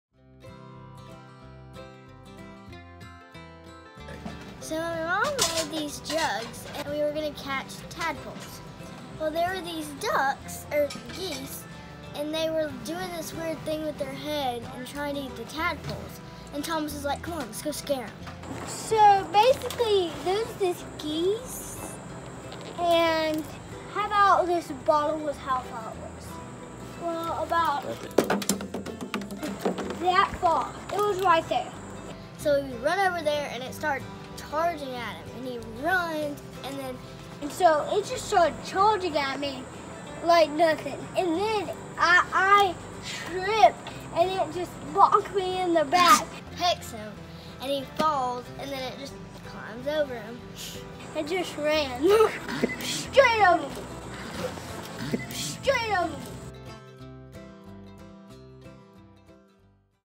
😂Hewett & Friends *Family fun Moment* "The Geese Story" 😂
Enjoy this fun story from our current outdoor family fun! It cracked me up so much!!

Chris Hewett
The Williams Group at Keller Williams Realty